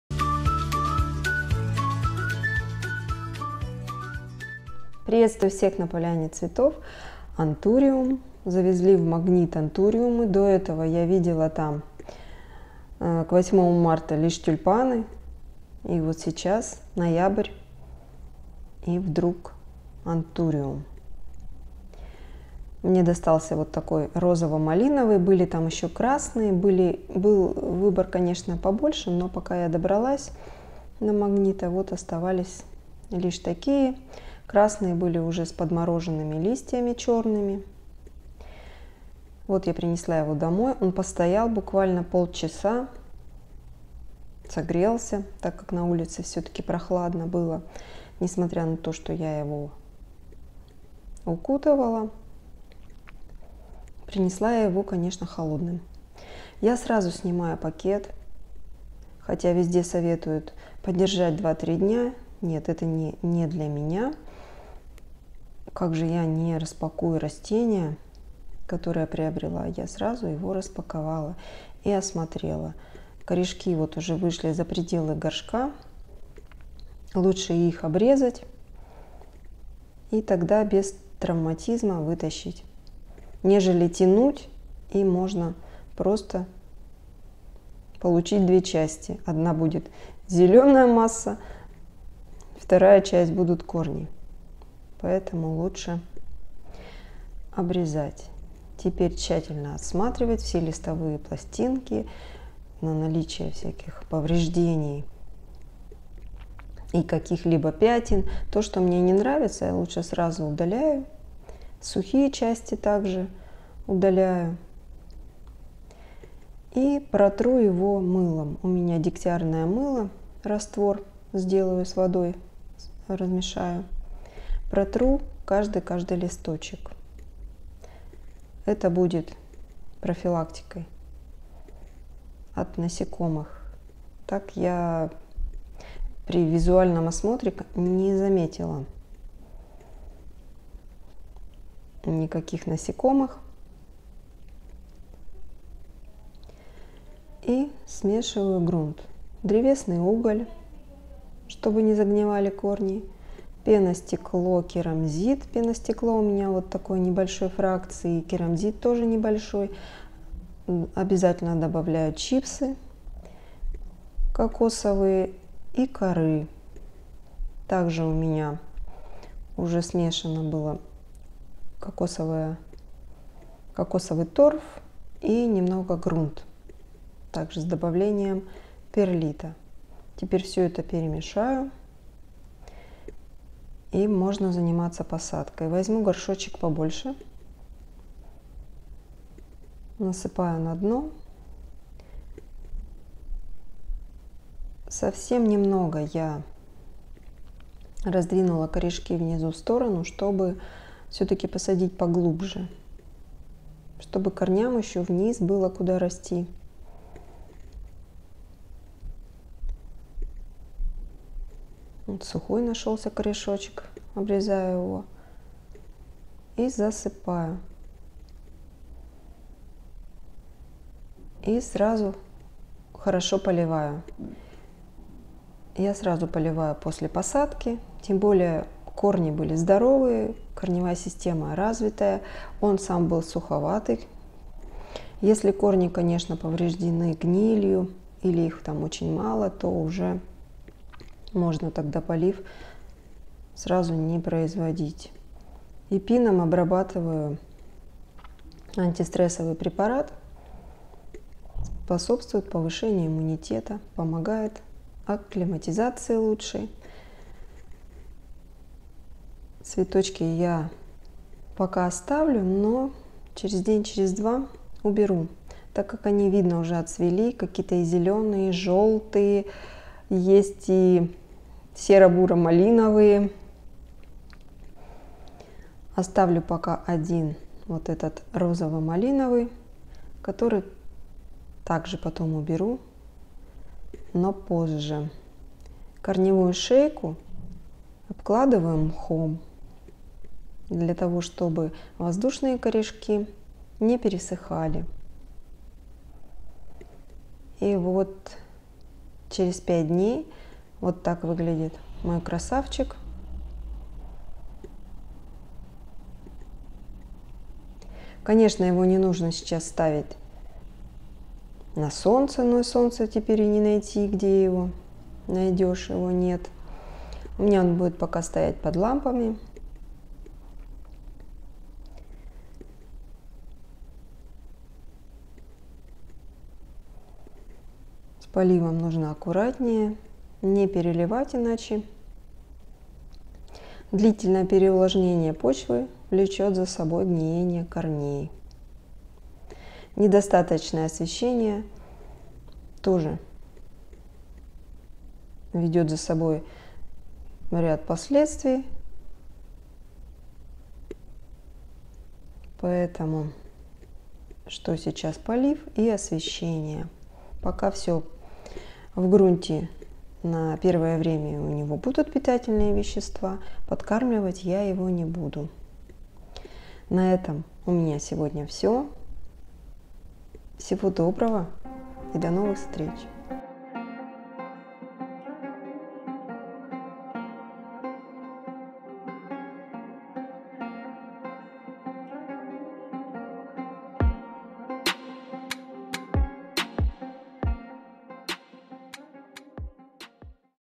АНТУРИУМ ИЗ МАГАЗИНА МАГНИТ*ПЕРЕСАДКА ПОСЛЕ ПОКУПКИ.
Антуриум после покупки.
Ещё видео